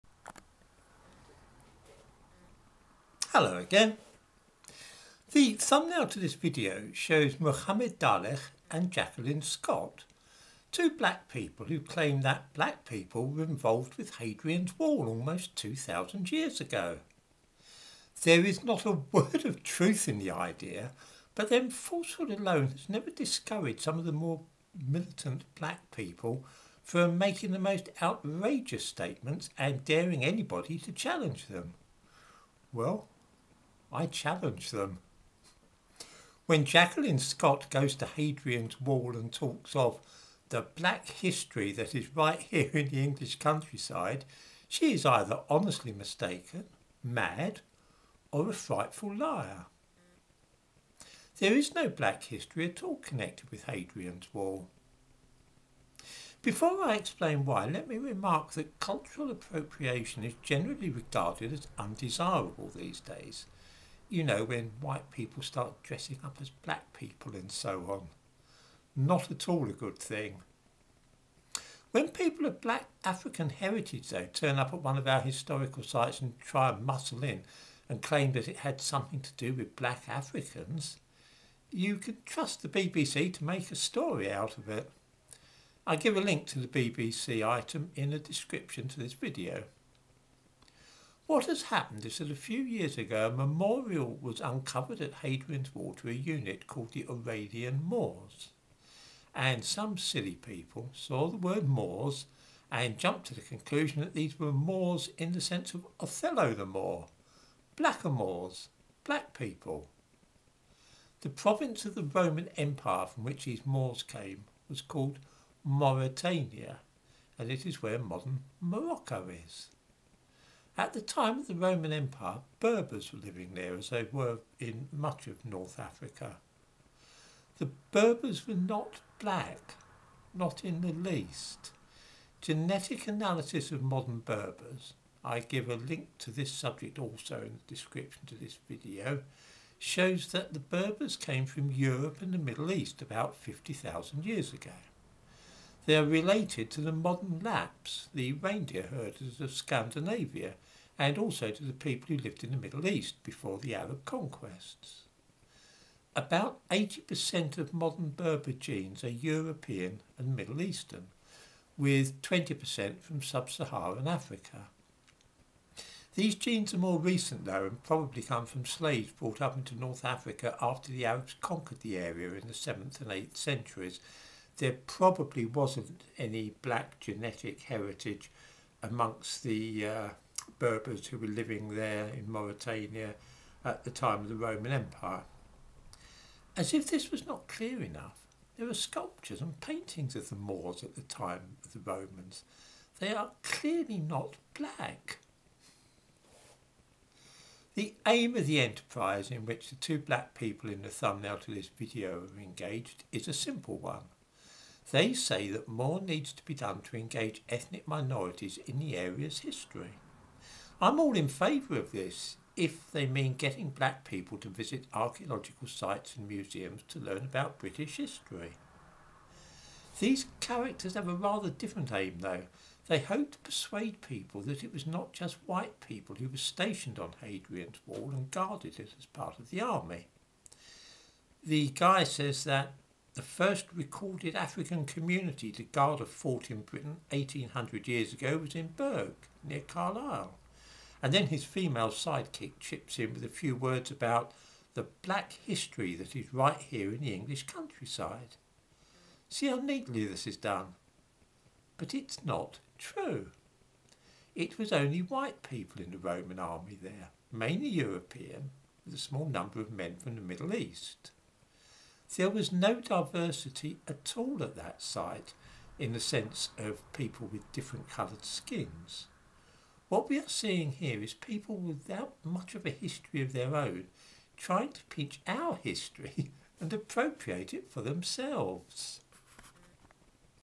An attempt at the cultural appropriation of British history by those of African origin
Cultural appropriation is frowned on these days, unless it is being carried out by black people trying to lay a claim to European history.